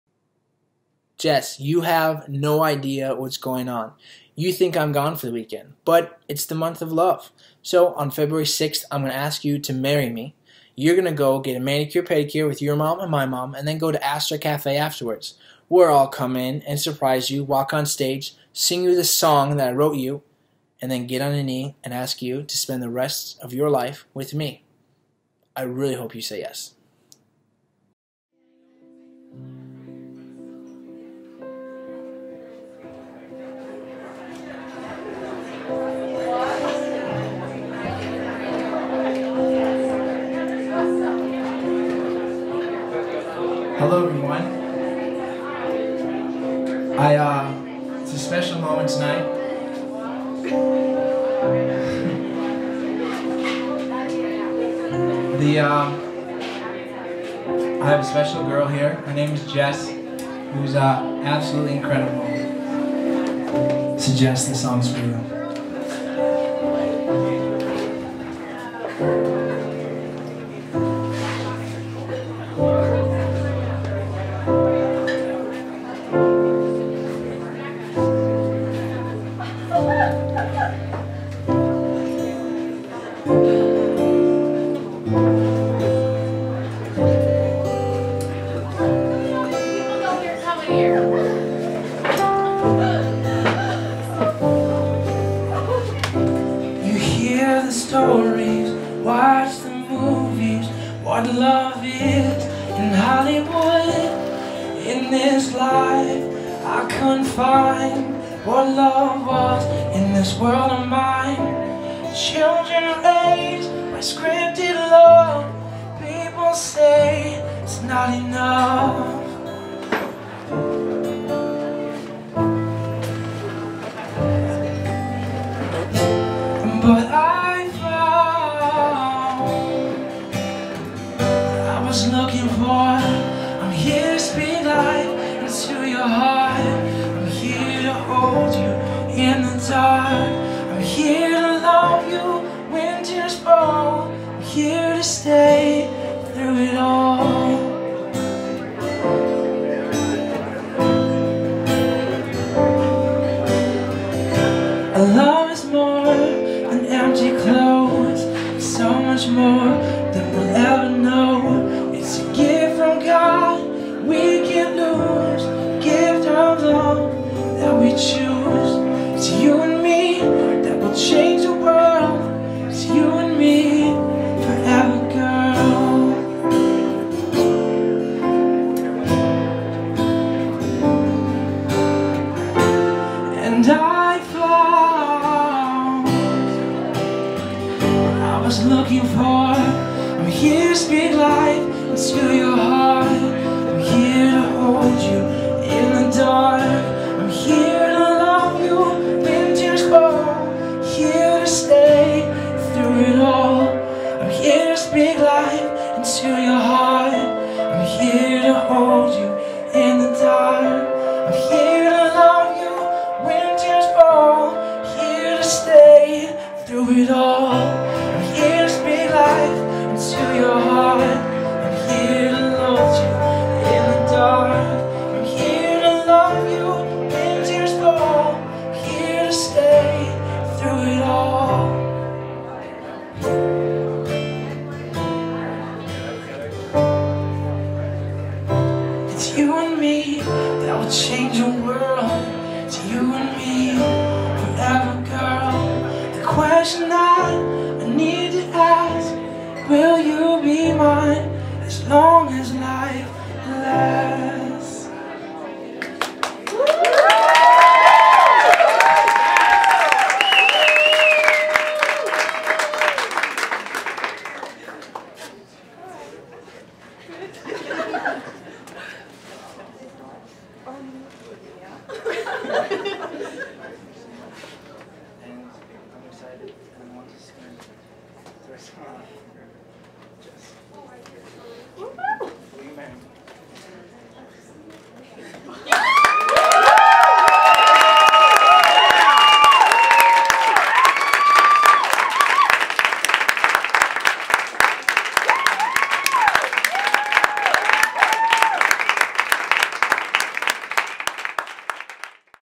My Surprise Proposal to My Best Friend.
My dream since I was a boy was to write a song for my future wife and sing it to her, then drop on a knee. So, Jess, her mother, and my mother got manicures then headed to Aster Cafe where myself, our families, and friends were hiding. After Jess and the moms sat down and got situated I walked onto the stage and sang my song to her.

Spoil ALERT: Jess said "absolutely” :)

p.s. I was a nervous wreck!